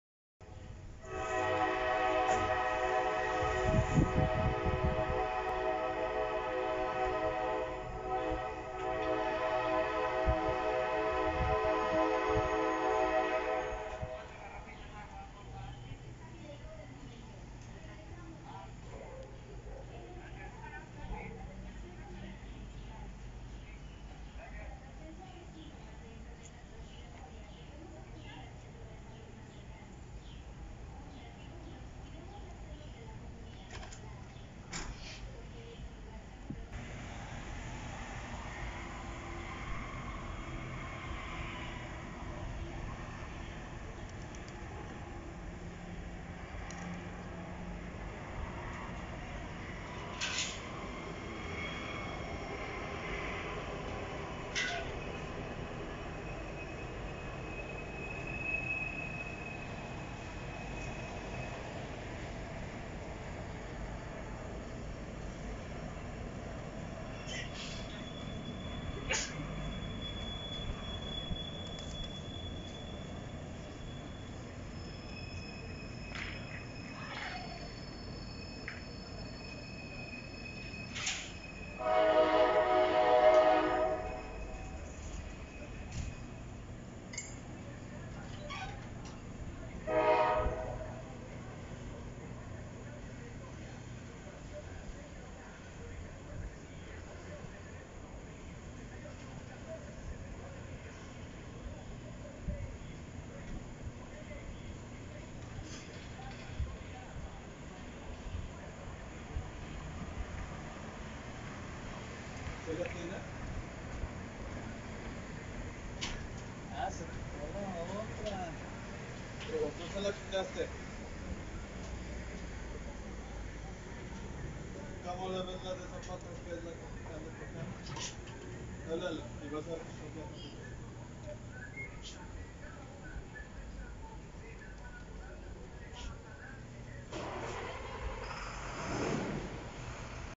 LXAVC de 4 locomotoras
Después de la llegada del las Ferrosur en la mañana; bajó este tren rumbo a Veracruz.